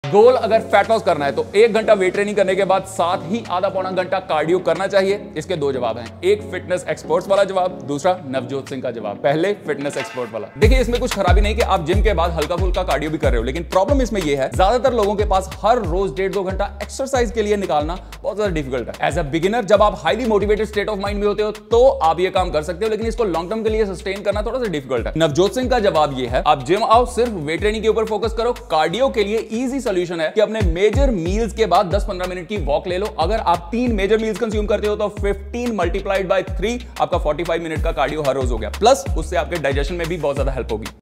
❌🥵 Gym + Cardio GOOD or BAD?
Should you do one hour weight training and one hour cardio to lose fat?